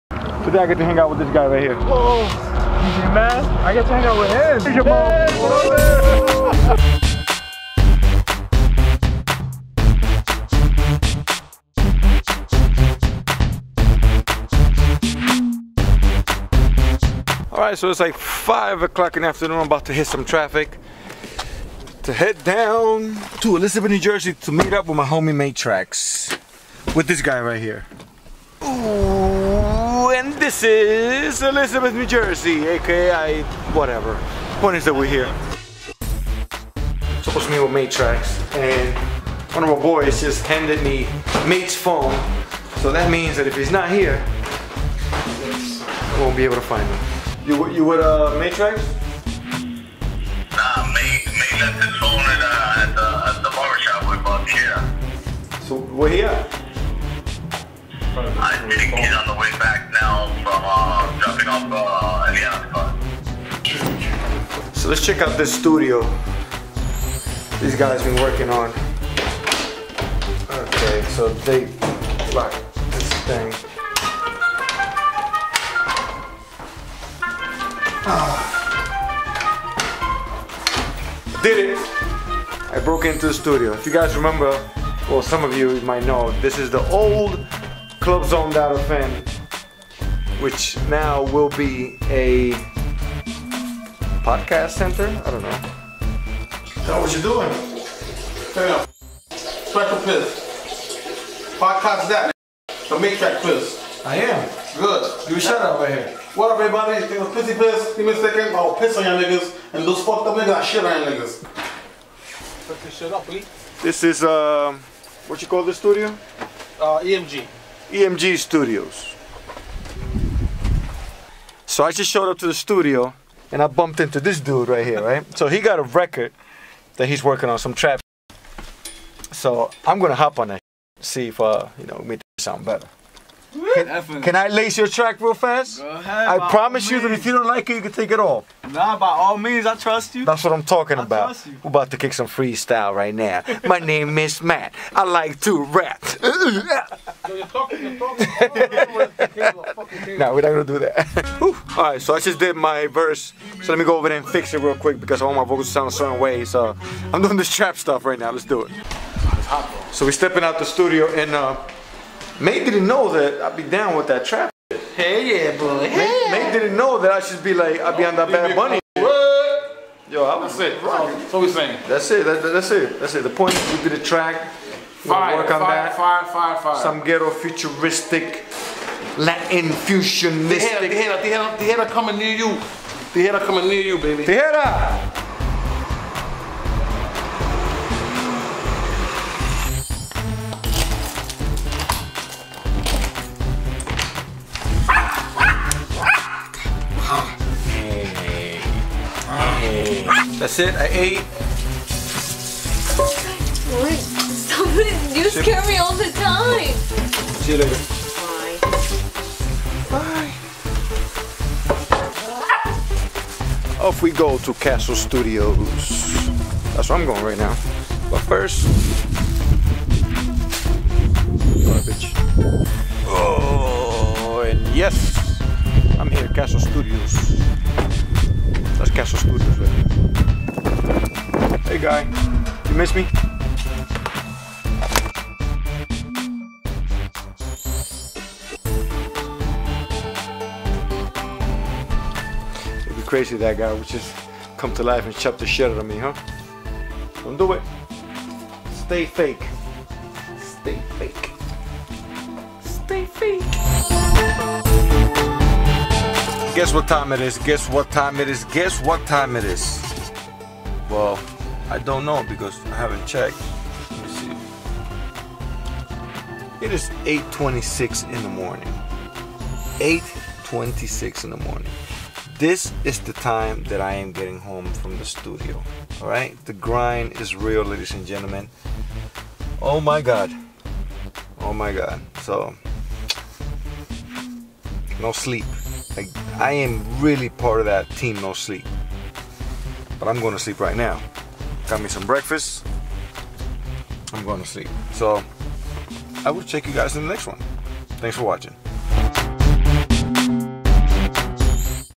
Busy like a bee (Vlog#053)
Here's a day in my busy life, running from the club, studio to studio.
Recording for myself and for Romeo Santos in Castle Studios. Also ended up at The Lobby in Elizabeth New Jrsey, Always a great party to play at. Stay tune as i will be doing more blogs more often and coming soon i will be doing daily blogs.

Remember that the best compliment is a simple share.